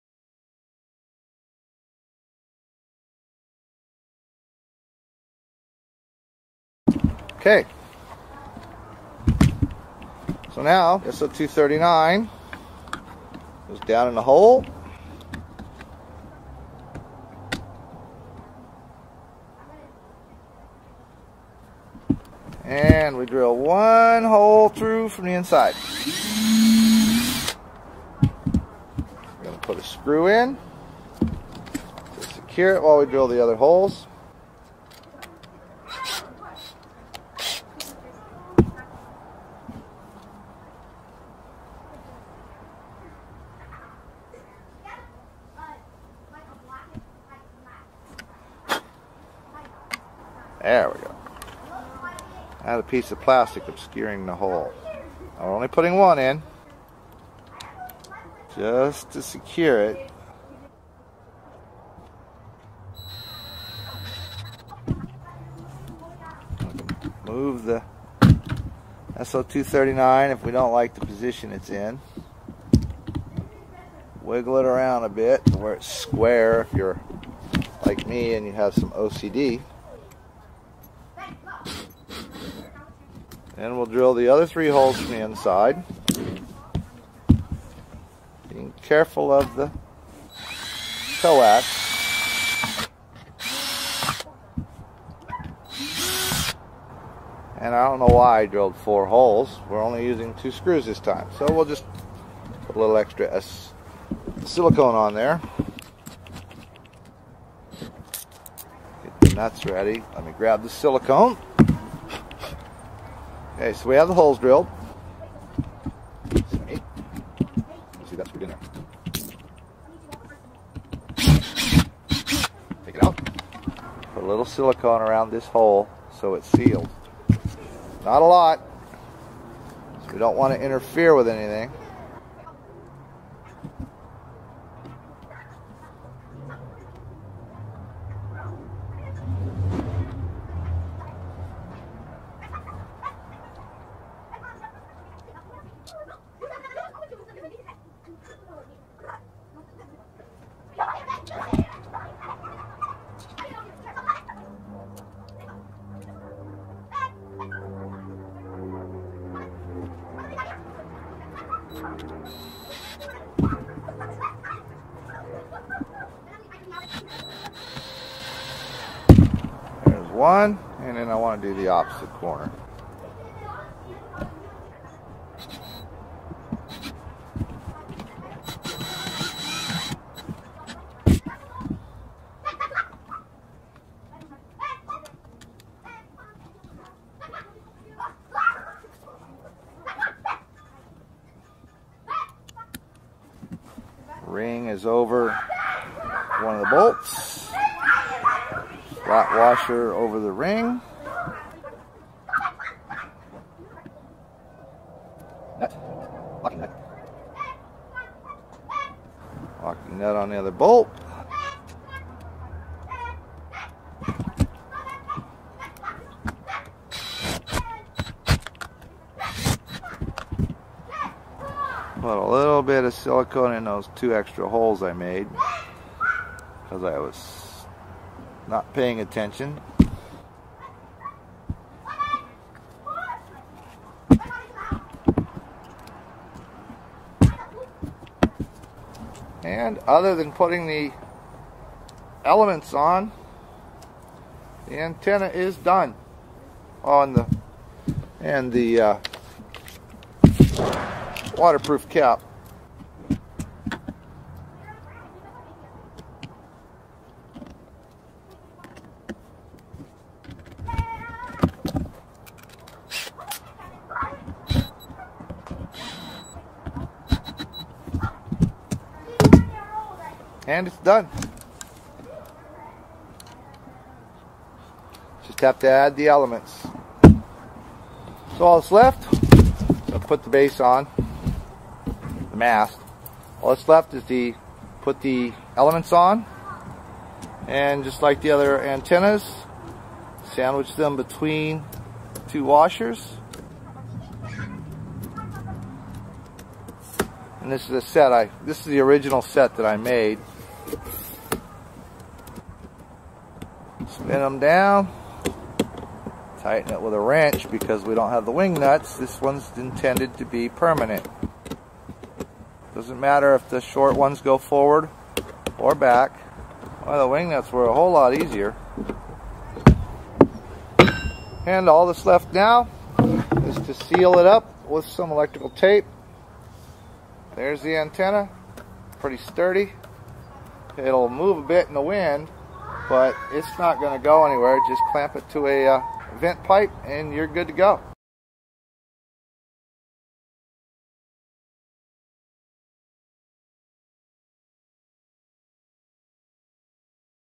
BUILD # 3 - Dual band vertical Dipole (Pt 3 of 3)
I build a version of my antenna for mounting on a vent pipe, mast, or other pole/post.

SWR was 1.4 and lower across the 2m band. Across the 70cm band it wasn't as good but was usable except for a narrow spike in one area of the band and even that might be useable as the highest SWR was 2.2 to 1

The lengths, measured from the outside of the bottom bend, are:
6 1/4" for the 70cm side
18 5/8" for the 2m side